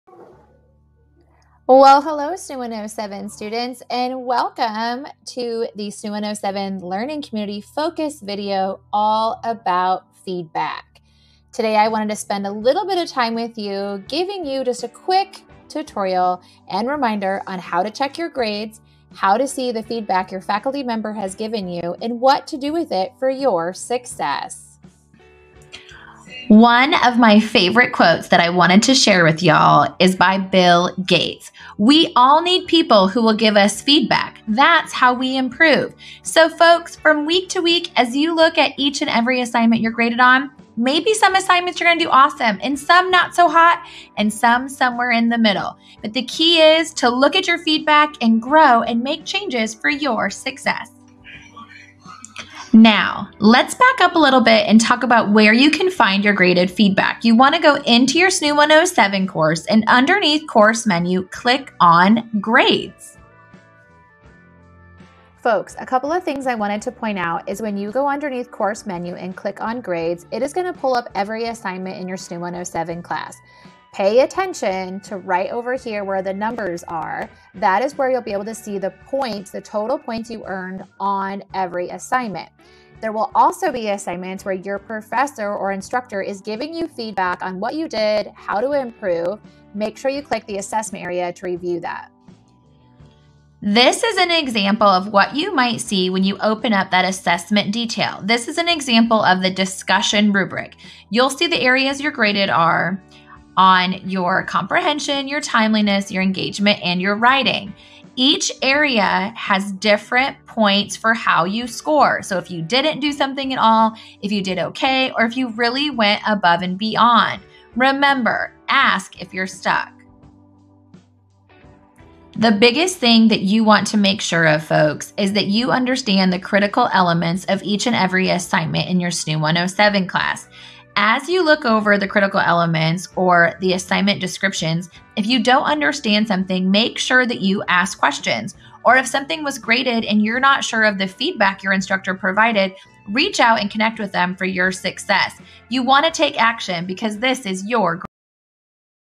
Week 6/Module 6: SNHU 107 LC FOCUS-Using Feedback
This video shows you how to find your faculty feedback in the grade center and WHY this is important for your success. Remember to reach out to your faculty with any questions as we are here to SUPPORT your SUCCESS!